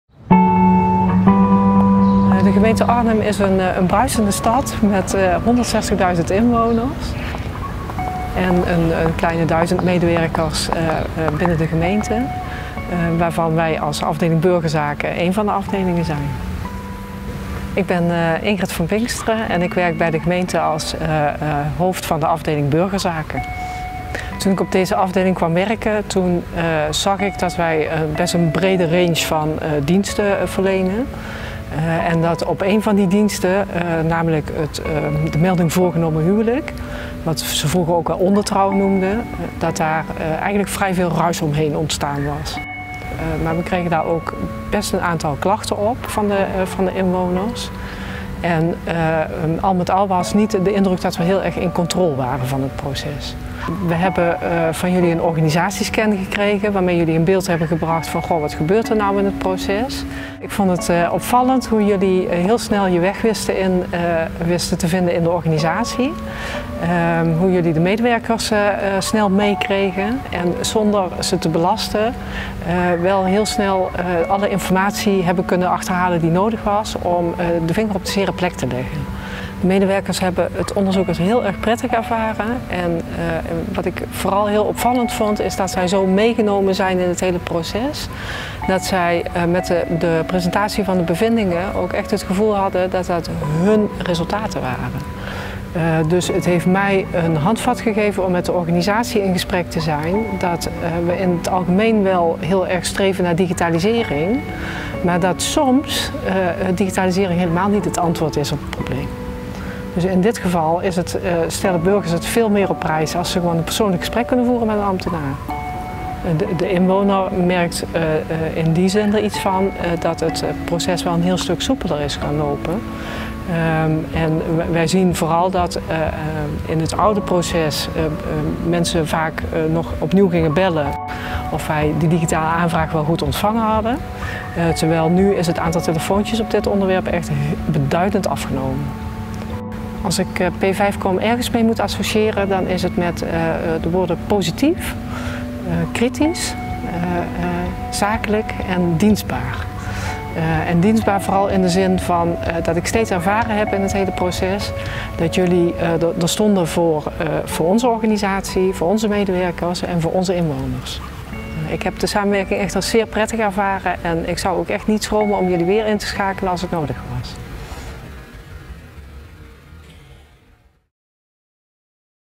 Trouwen zonder ruis dankzij heldere processen in Arnhem | Casus
Trouwen is een bijzondere stap in een mensenleven. Het melden van een voorgenomen huwelijk moet soepel gaan. De gemeente Arnhem merkte dat dit niet altijd het geval was. Samen met de medewerkers bracht P5COM het hele proces in kaart en legden we de focus op de inwoners. Wat merkten zij van de dienstverlening van de gemeente en wat kon er beter? Vanuit het gezamenlijke beeld van de ambtenaren en de inwoners begon de optimalisatie. Het resultaat? Minder klachten, minder telefoonverkeer en enthousiaste bruidsparen, die zich gehoord voelen.